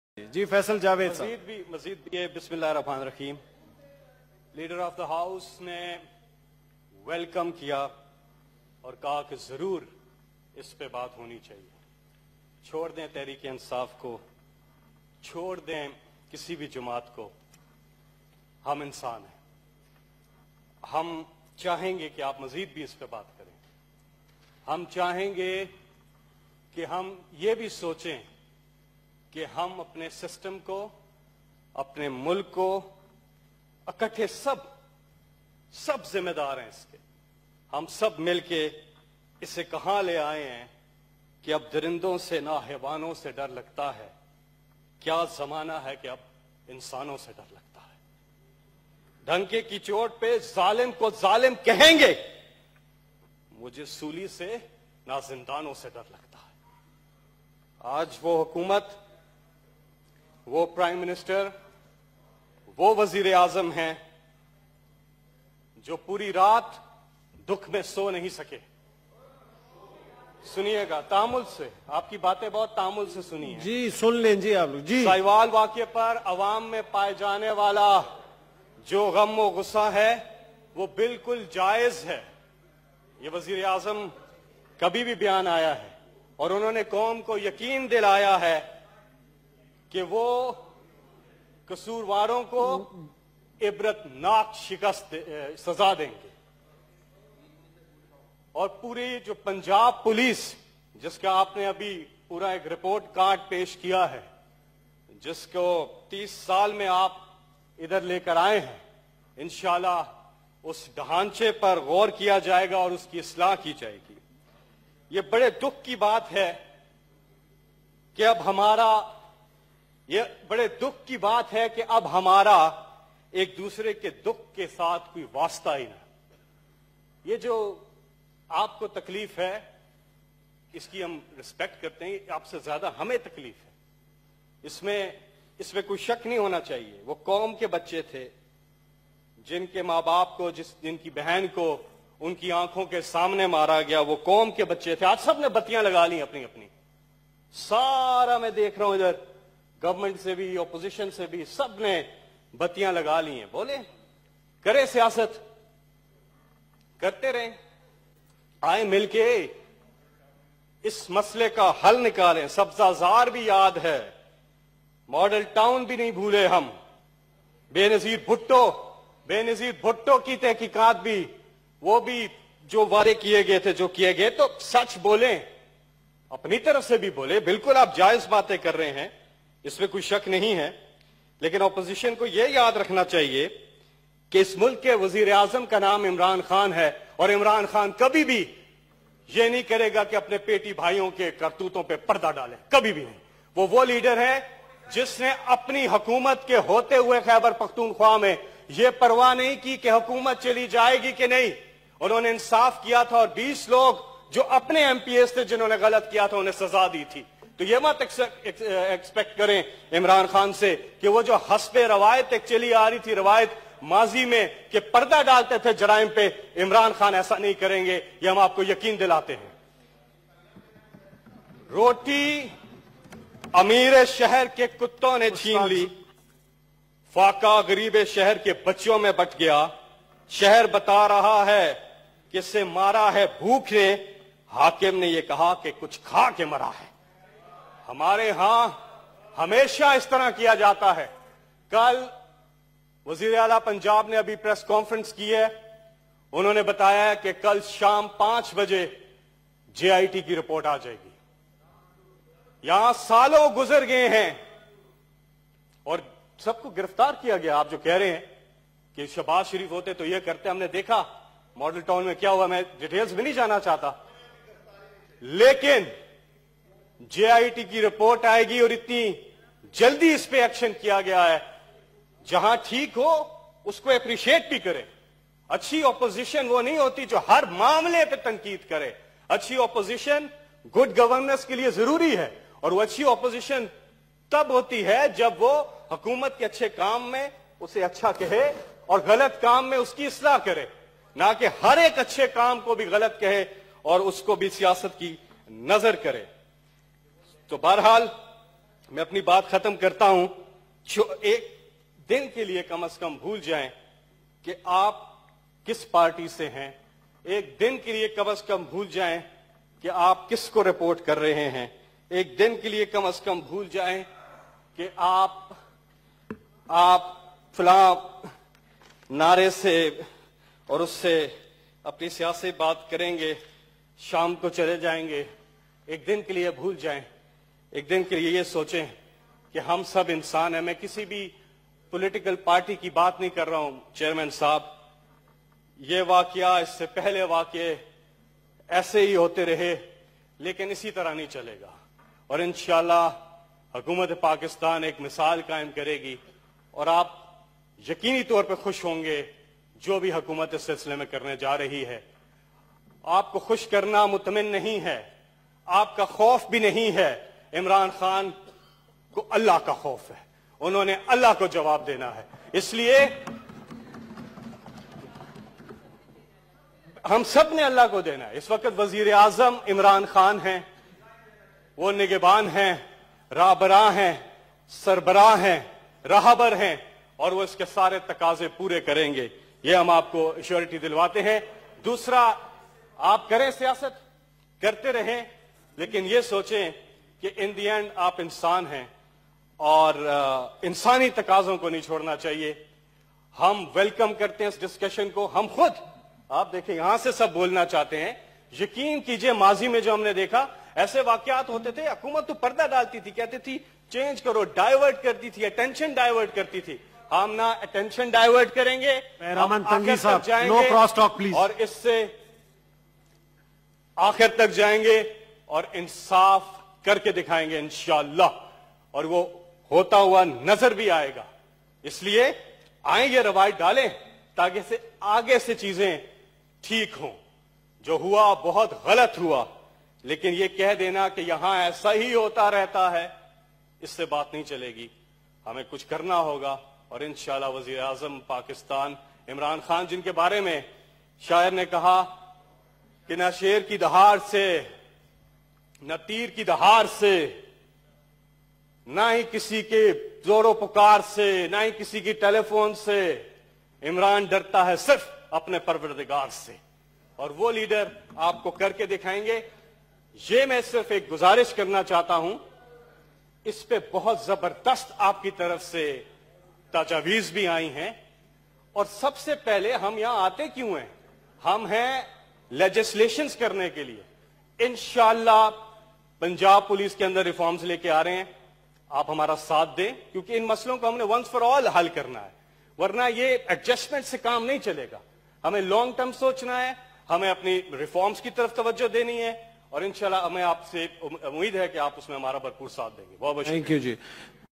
Senator Faisal Javed Khan Speech Senate of Pakistan Islamabad (21.01.19)
PTI Official YouTube Channel